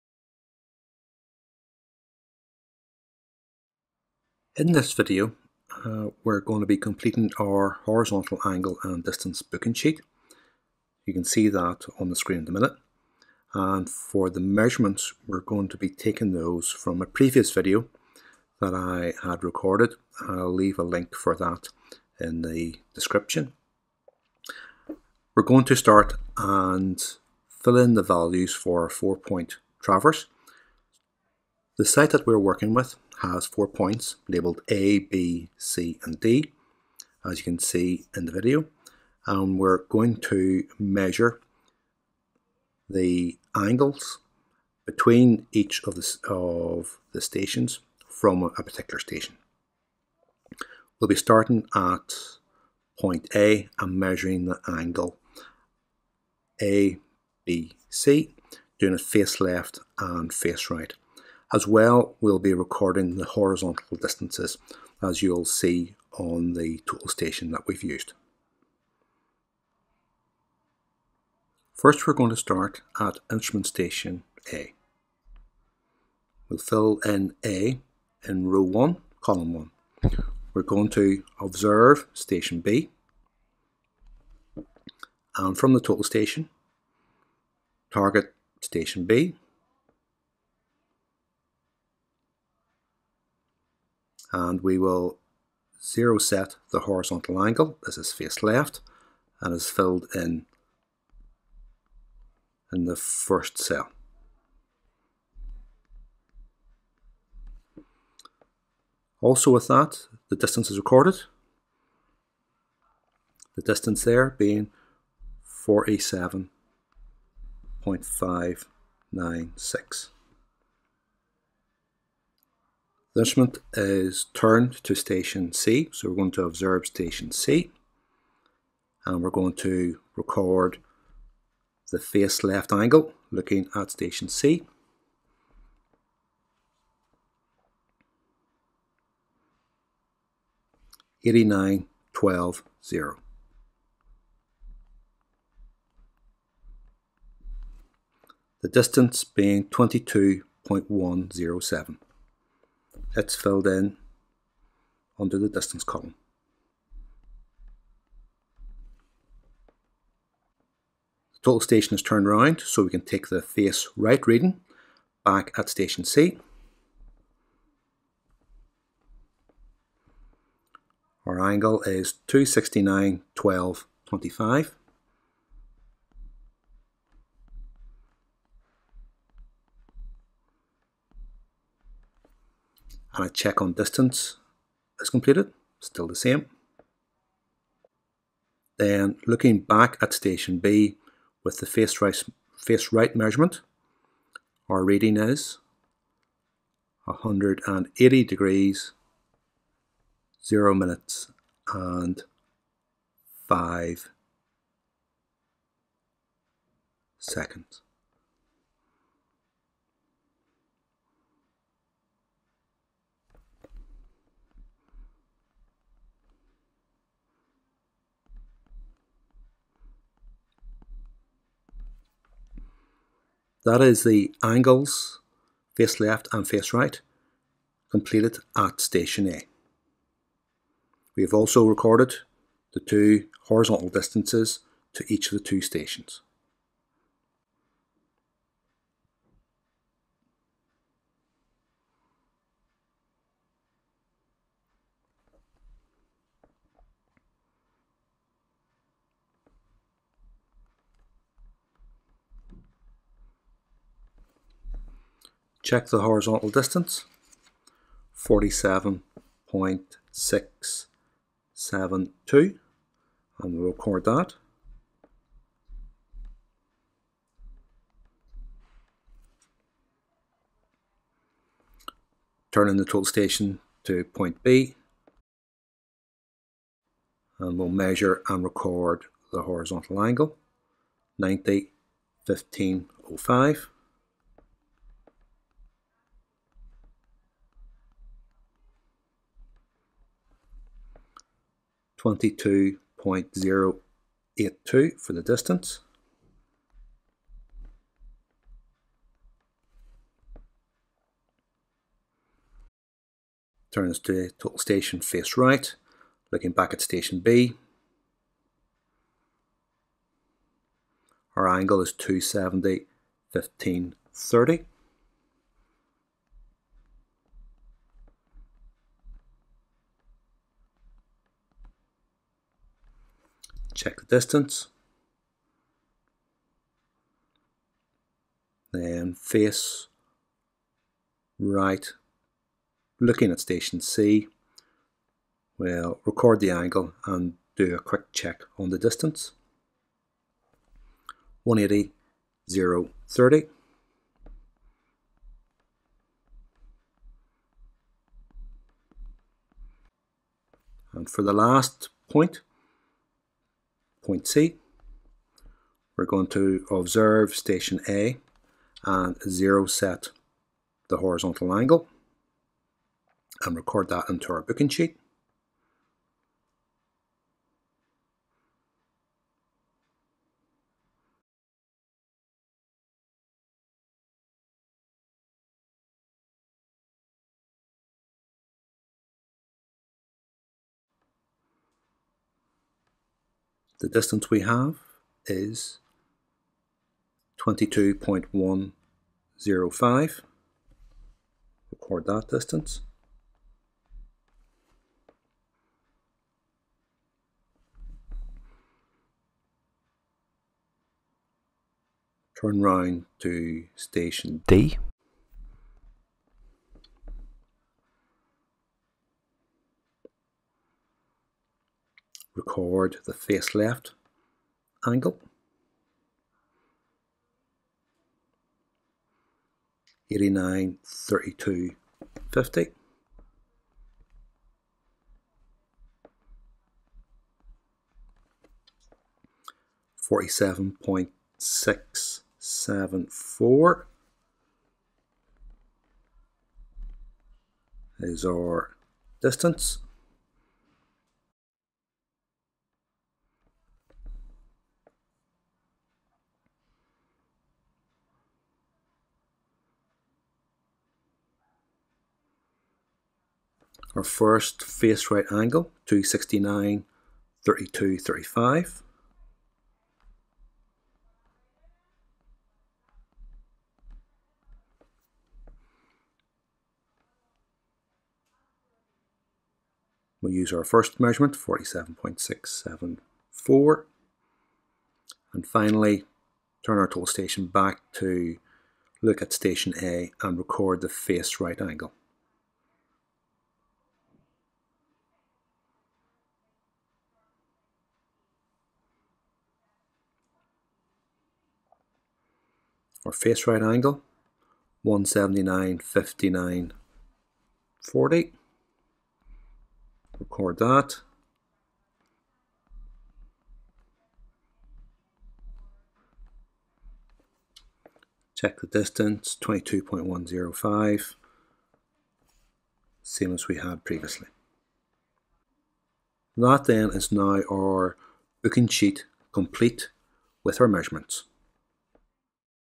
4 Point Closed Traverse Part 1 - Recording measurements by Total Station
This video covers the recording of Face Left, Face Right angles and Horizontal Distances between stations during a 4 Point Closed Traverse.